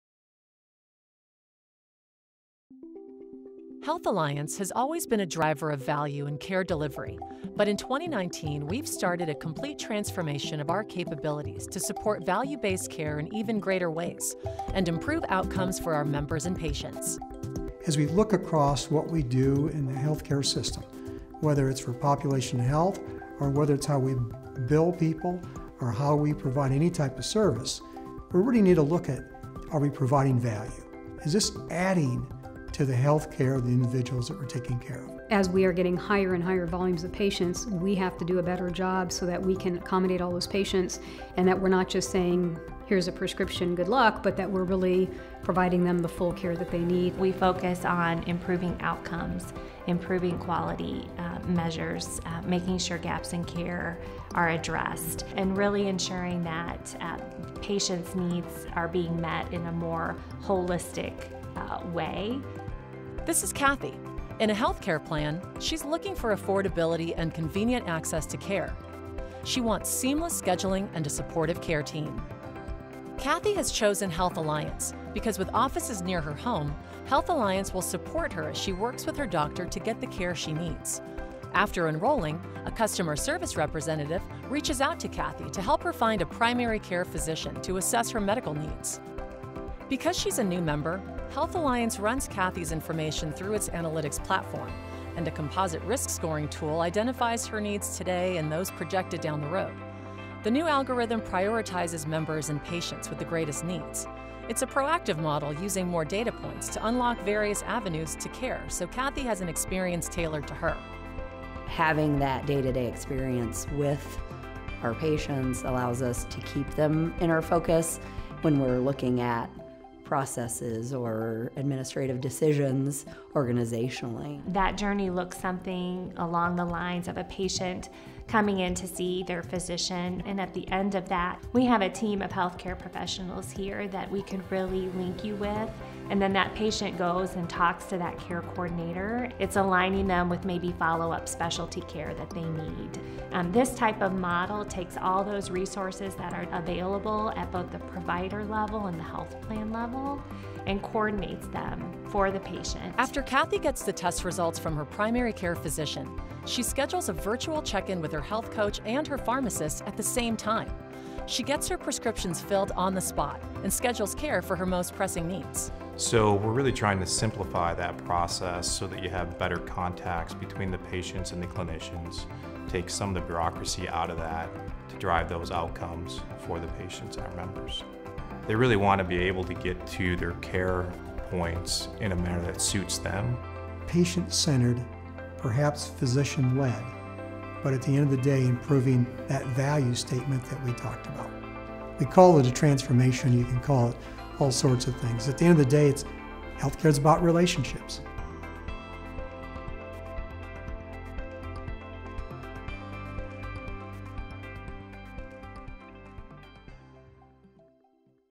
Reshaping Care at Health Alliance and Carle
Population health is reshaping care at Health Alliance and Carle. It's about identifying and customizing care for patients and members who can benefit from an enhanced experience. This results either in helping them stay well or helping manage a chronic condition. Carle is putting new population health initiatives into practice under what we’re calling the Reshaping Care umbrella. Even better, we’re learning along the way from Health Alliance, which has been supporting its members like this for decades.